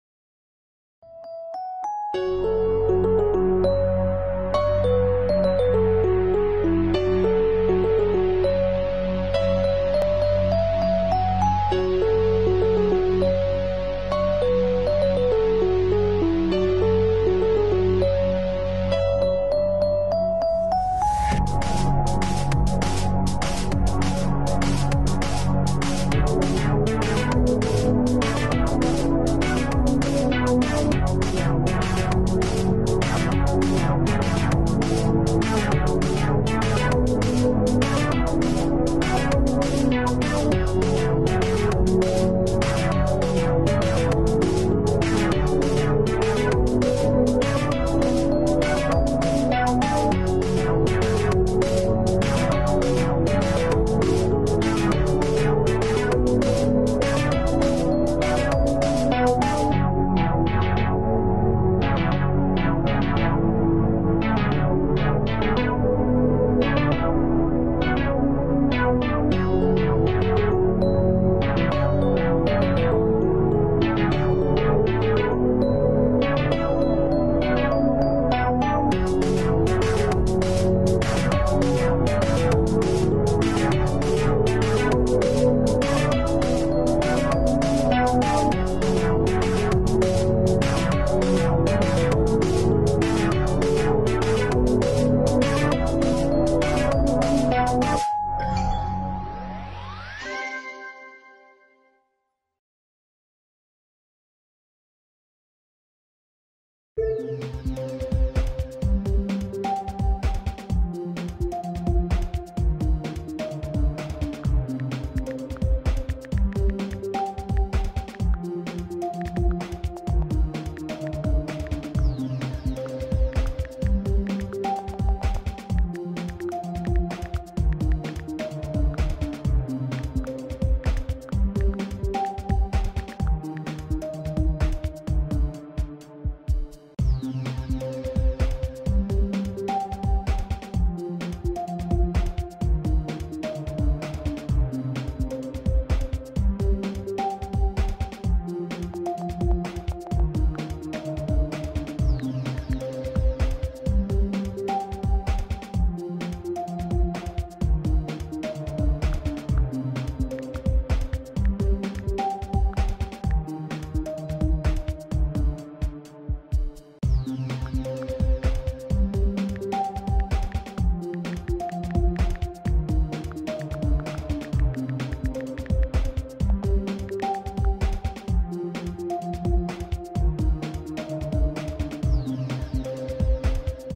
(My 150th demon) TOE III (Hard Demon) by Manix648
I started this level over a year ago, and I finally came back and finished it and now I have 150 demons!

Where do I begin with this level. This level is beyond a masterpiece, its one of my favorites for sure. This level is so unique and the gameplay is phenomenal. If you are going for a hard demon, go for this. I have a pretty big project next, which I will reveal soon.

Attempts: 2628 (of course, a LOT were old)
Enjoyment: 10/10